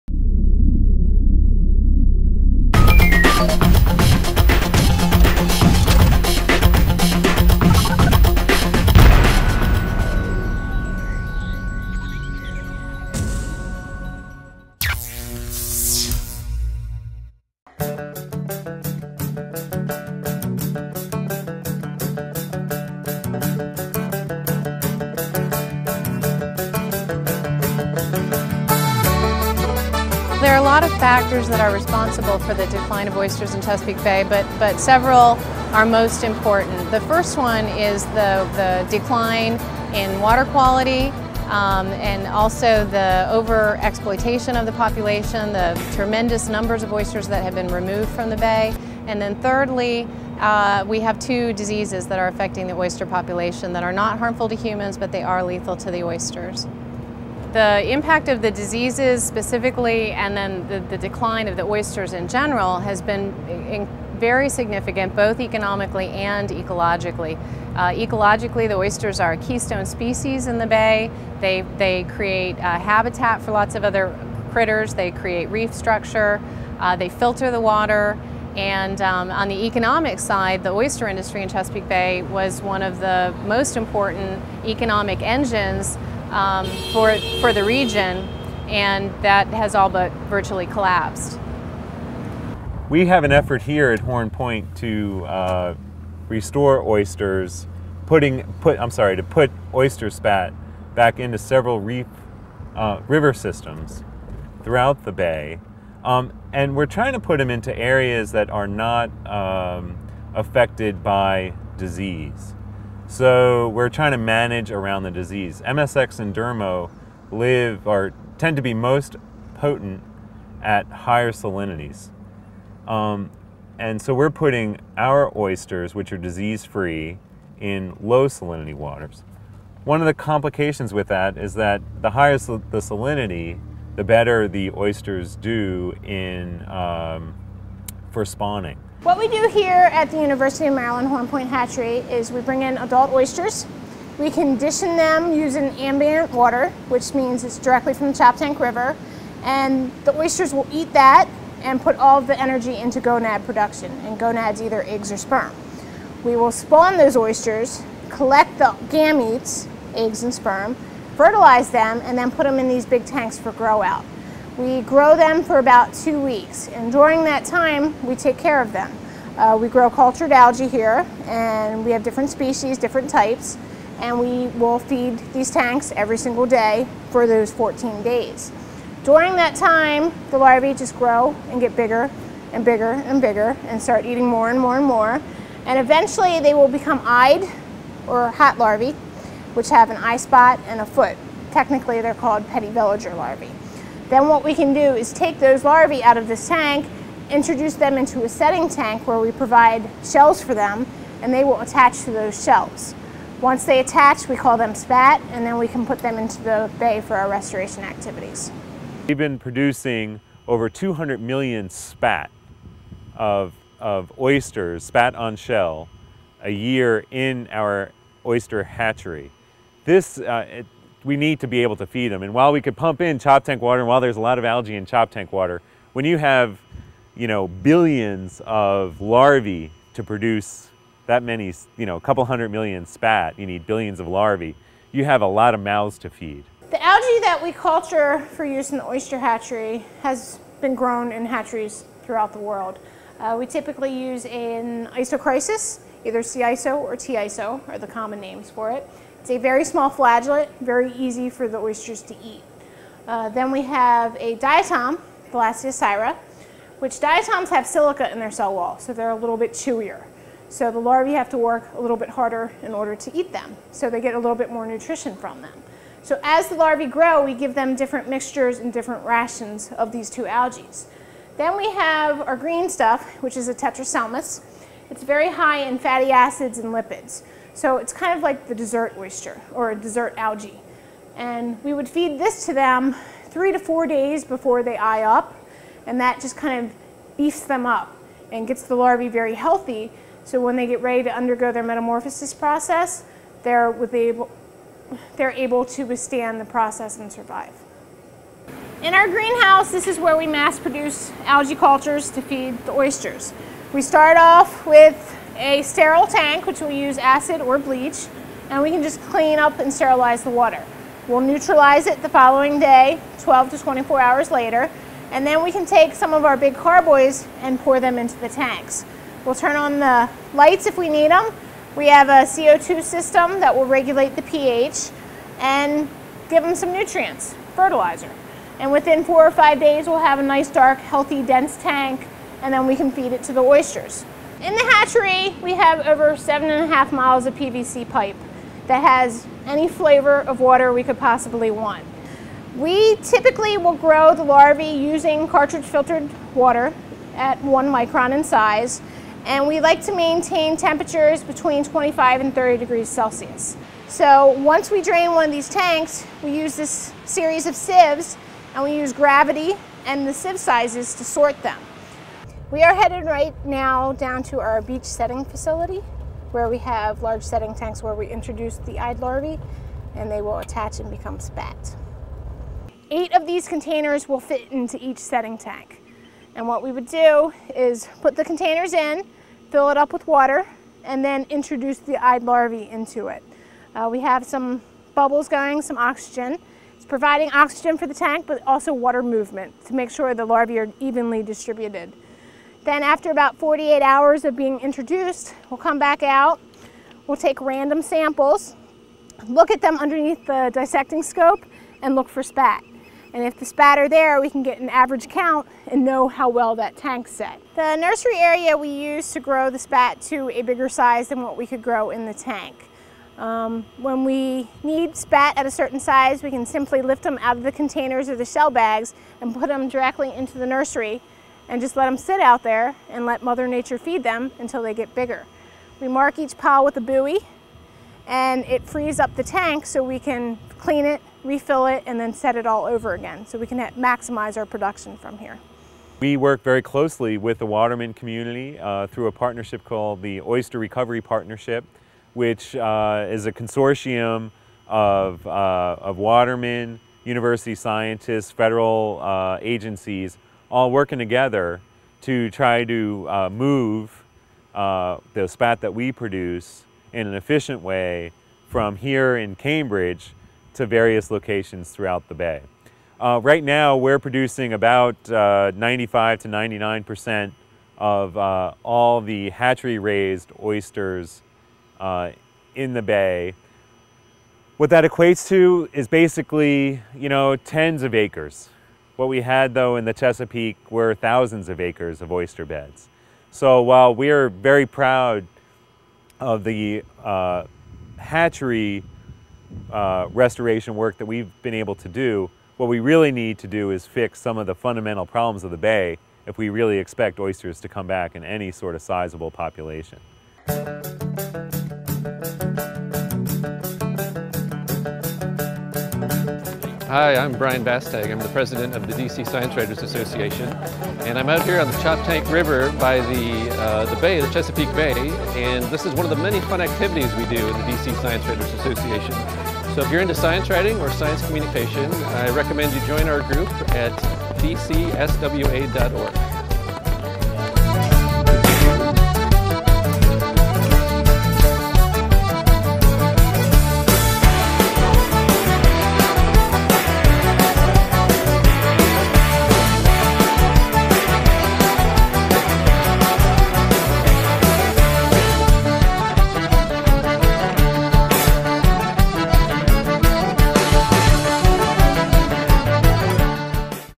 Save the Oysters - Part 1 (MWV9)
MicrobeWorld looks at the impact of disease on the Chesapeake Bays oyster population and how scientists are using cultured algae to restore them.